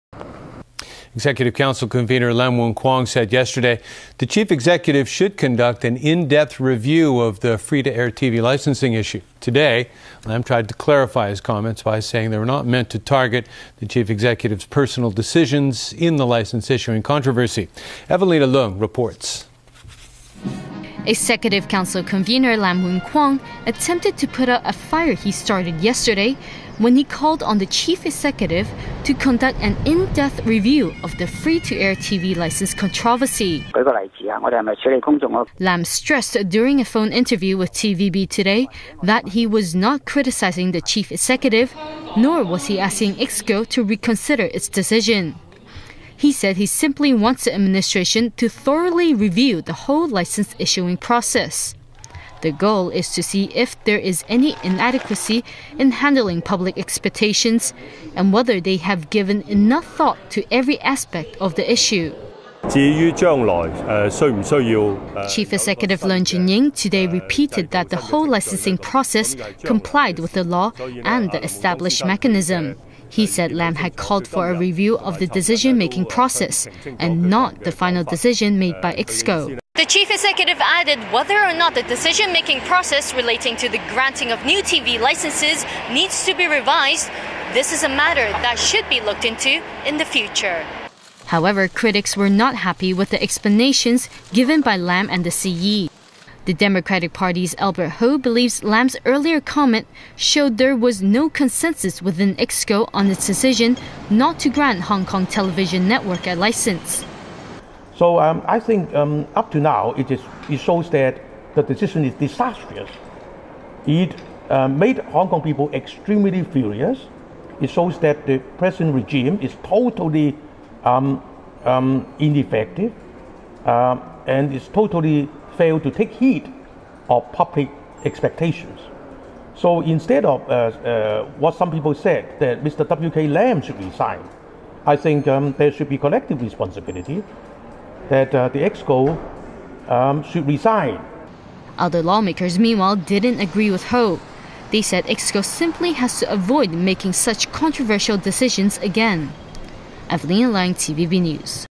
11-11-2013 | Evelina Leung | Lam Woon-kwong claimed his words aren't targeting CY Leung
林煥光澄清其言論並非針對梁振英